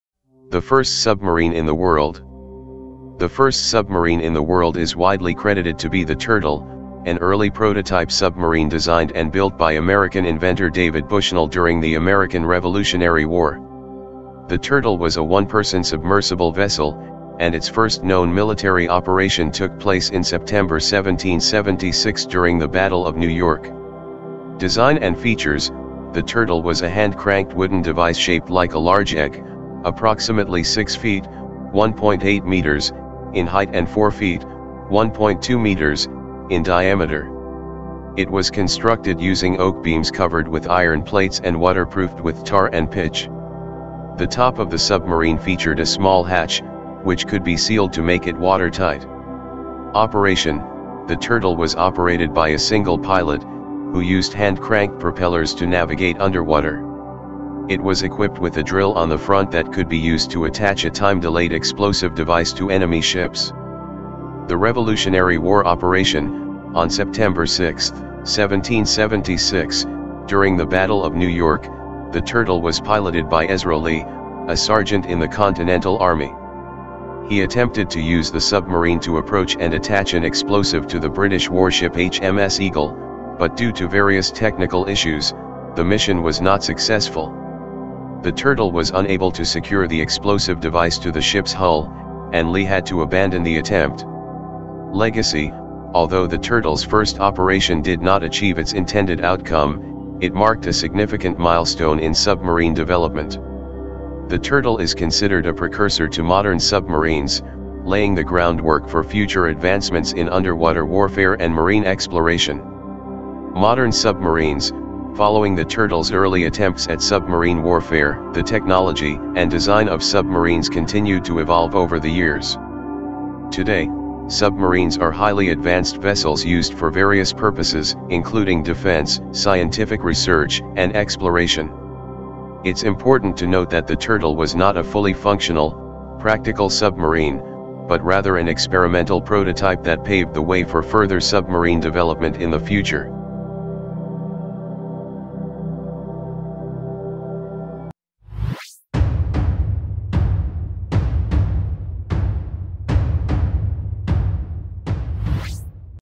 The First Submarine in the World
Explore the pioneering marvel of the world's First Submarine: an engineering feat that revolutionized naval warfare. Unearth its historic significance and the visionary minds behind its creation. Discover a new era of underwater exploration and defense with this groundbreaking invention.